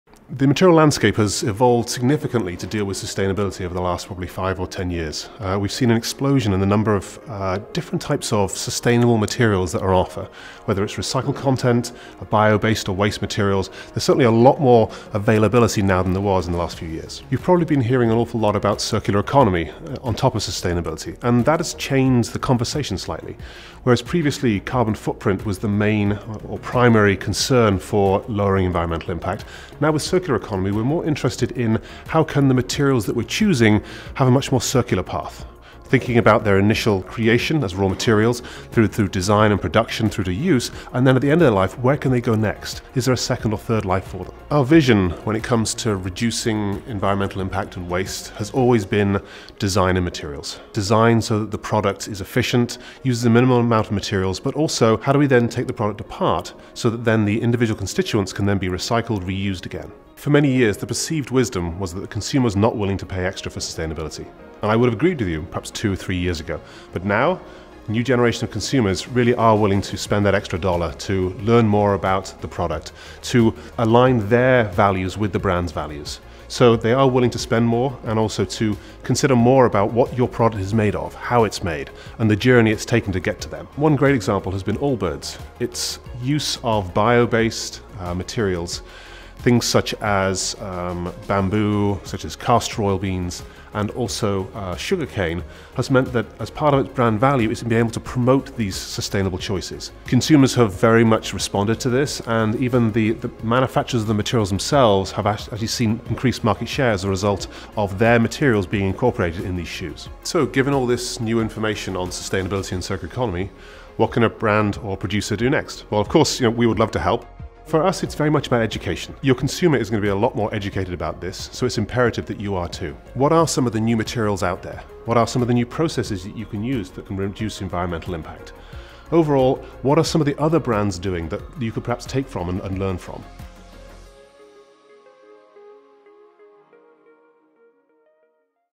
Eye On: Sustainability, Circular Economy, and the Consumer Mindset
Sustainability has been top of mind for some time now, but the conversation is shifting to include the the concept and methods found within circular economy.

Dr. Andrew Dent talks about adding circularity into our products: from raw materials, through design and production to use, and then where the product or its parts can go next.